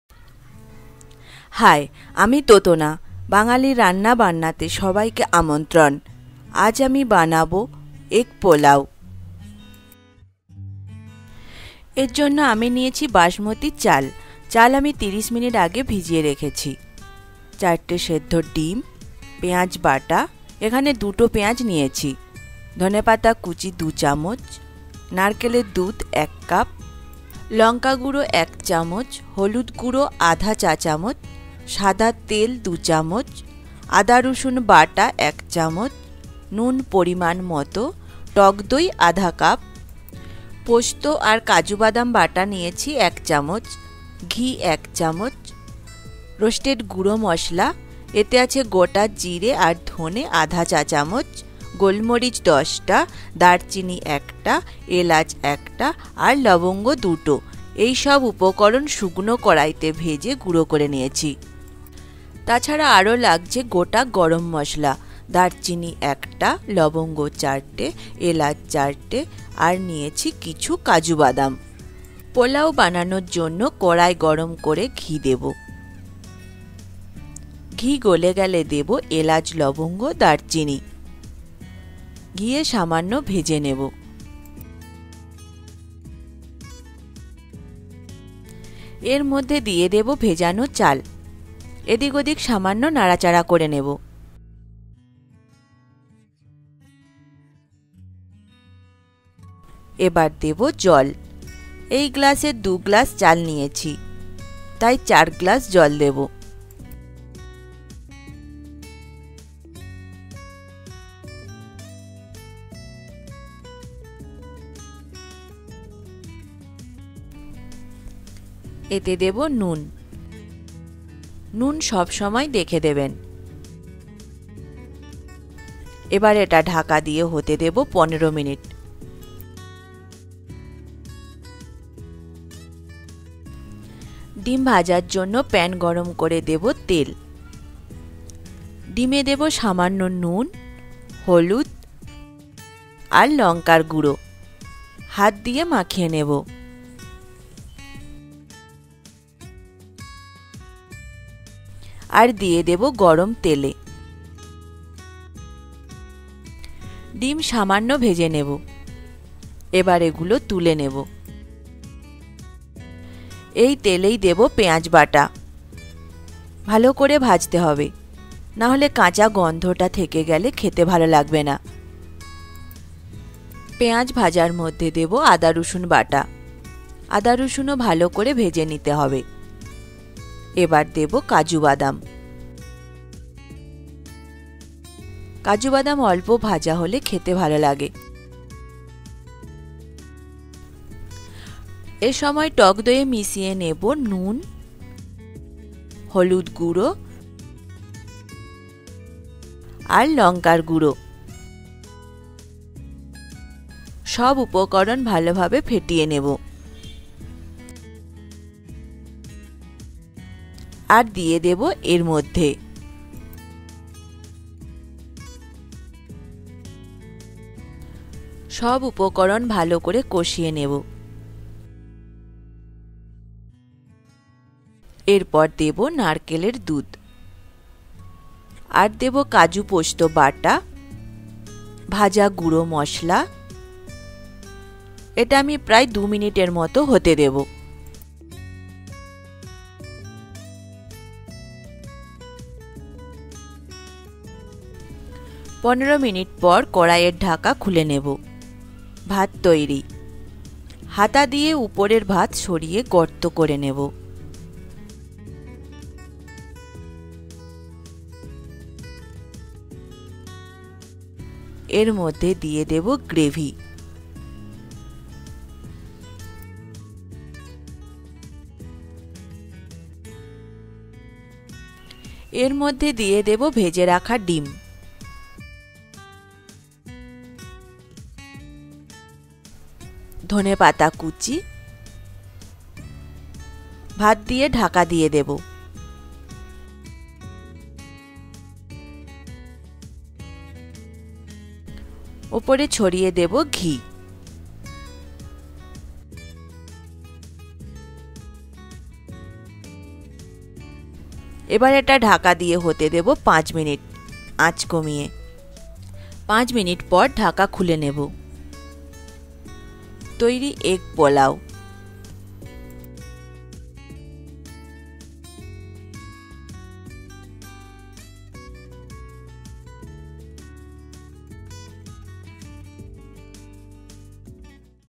Egg Pulao Recipe | Easy Pulao Recipe | How to make Egg Pulao
How to make easy Egg Pulao  Recipe? Egg Pulao  Recipe is the popular Indian dish made with boiled egg and rice. Learn how to cook Egg Pulao?

Ingredients for Egg Pulao Rice Recipe:
Basmati Rice 350 grams, soaked in water for 30 minutes
Boiled Egg Four
Onion paste 2 tsp
Chopped Coriander leaves 2 tsp
White oil 2 tsp
Poppy seeds and Cashew nut paste 1 tsp
Fried Roasted Powder (Cumin and Coriander ½ tsp, Black Peeper 10, Cinnamon 1, Cardamom 1, Cloves 2) Fry all the spices in a pan without Oil and make it powder.